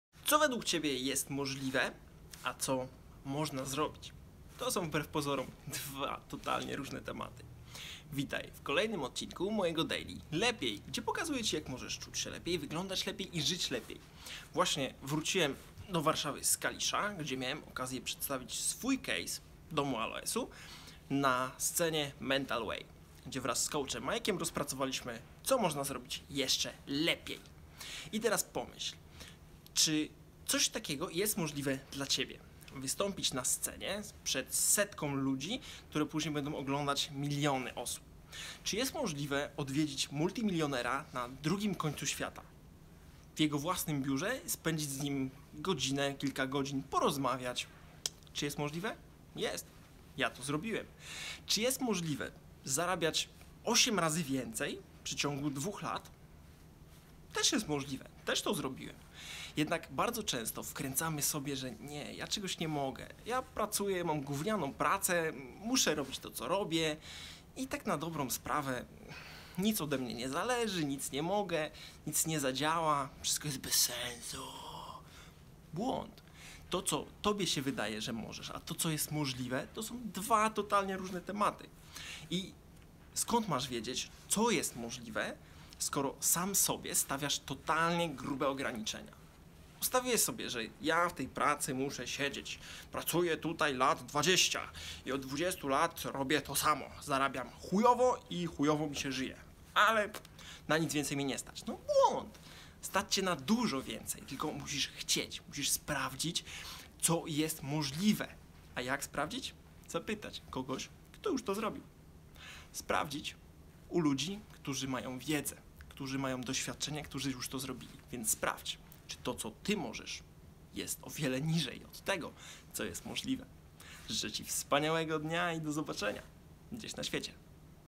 Daily LEPIEJ - dzień 95: Co możesz, a co jest możliwe?
Daily Vlog
Znajdziesz tu 101 sposobów na to jak Czuć się, Wyglądać i Żyć

Życzę Ci wspaniałego dnia i do zobaczenia gdzieś na świecie!
Paweł Rekowski - Kreator Uśmiechu

Co, według Ciebie, jest możliwe, a co można zrobić? To są, wbrew pozorom, dwa totalnie różne tematy.

Witaj w kolejnym odcinku mojego daily Lepiej, gdzie pokazuję Ci, jak możesz czuć się lepiej, wyglądać lepiej i żyć lepiej.

Właśnie wróciłem do Warszawy z Kalisza, gdzie miałem okazję przedstawić swój kejs Domu Aloesu na scenie Mental Way, gdzie wraz z coachem rozpracowaliśmy, co można zrobić jeszcze lepiej. Pomyśl teraz, czy coś takiego jest możliwe u Ciebie? Wystąpić na scenie przed setką ludzi i przedstawić coś, co później będą oglądać miliony osób. Czy jest możliwe odwiedzić multimilionera na drugim końcu świata, w jego własnym biurze, porozmawiać z nim godzinę czy kilka godzin? Jest to możliwe, ja to zrobiłem. Czy jest możliwe zarabiać 8 razy więcej w przeciągu 2 lat? Też jest możliwe, też to zrobiłem. Jednak bardzo często wkręcamy sobie, że czegoś nie możemy. Mamy gównianą pracę, musimy robić to, co robimy i tak na dobrą sprawę nic od nas nie zależy, nic nie możemy, nic nie zadziała i wszystko jest bez sensu. Błąd. To, co Tobie się wydaje, że możesz, a to, co jest możliwe, to są dwa totalnie róże tematy. Skąd masz wiedzieć, co jest możliwe, skoro sam sobie stawiasz totalnie grube ograniczenia? Ustaliłeś sobie, że musisz siedzieć w tej pracy, pracujesz tam powiedzmy 20 lat i od początku robisz to samo, zarabiasz chujowo i chujowo żyjesz. To błąd. Stać Cię na dużo więcej, tylko musisz chcieć, musisz sprawdzić co jest możliwe. A jak to sprawdzić? Zapytać kogoś, kto już to zrobił. Sprawdzić u ludzi, którzy mają wiedzę i doświadczenie, którzy już to zrobili. Sprawdź, czy to, co Ty możesz jest o wiele niżej od tego, co jest możliwe. Życzę Ci wspaniałego dnia i do zobaczenia gdzieś na świecie.